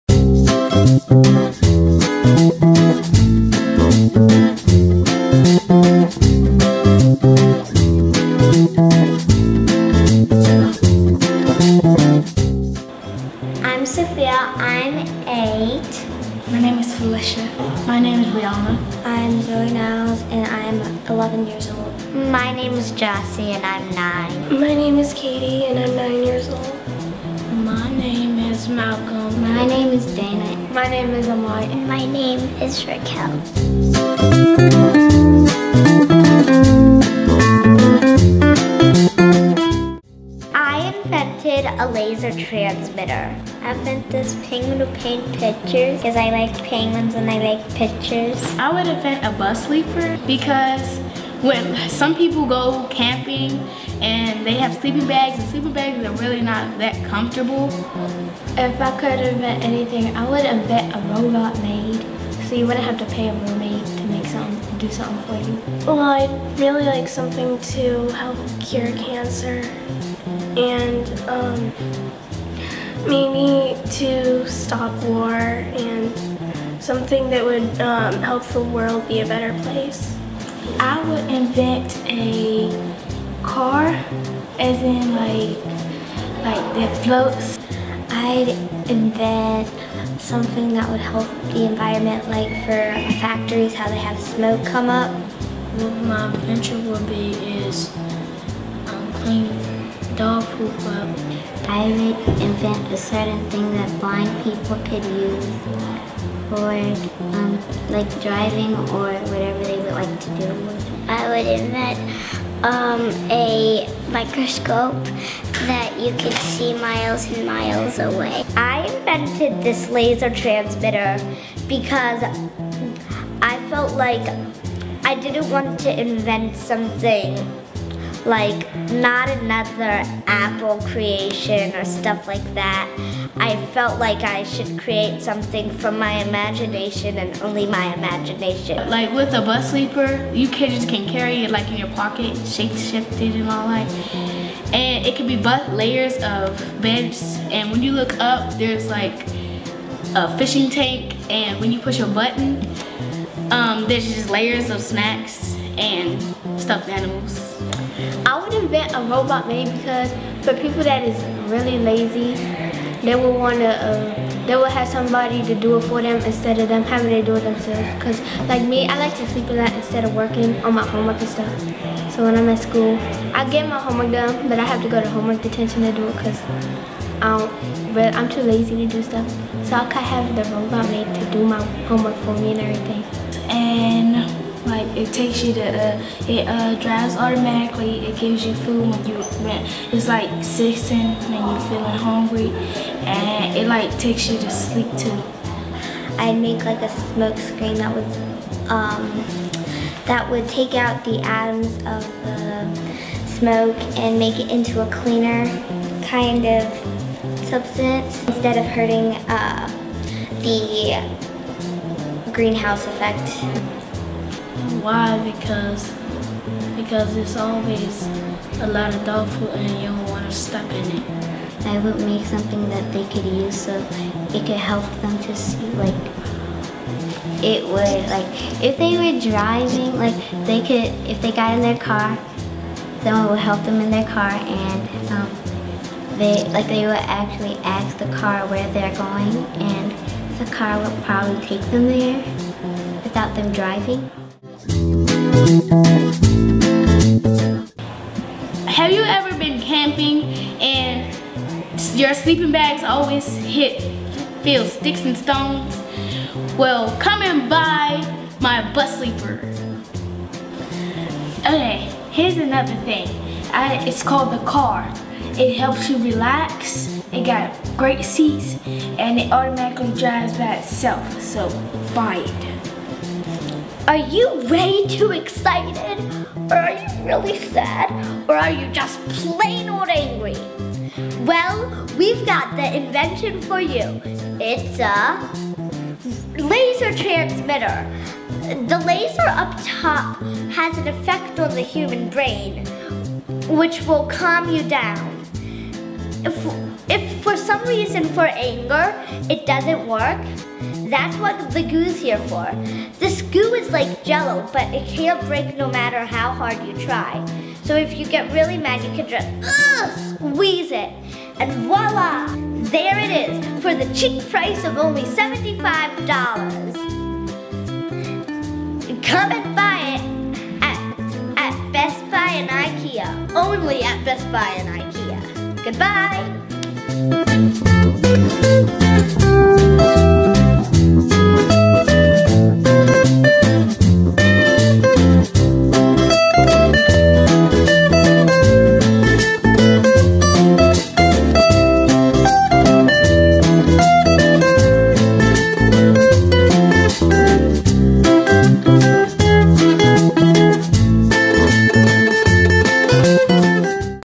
"Inventions" - Women's History Month Family Day 2012 Video Interviews
During the 2012 Women's History Month Family Day: A Celebration of Innovation and Invention, held at the Smithsonian National Portrait Gallery, a team of ARTLAB+ teens filmed video interviews, asking festival goers about what they would like to invent. To learn more about ARTLAB+ Production Teams and other programs, visit artlabplus.si.edu.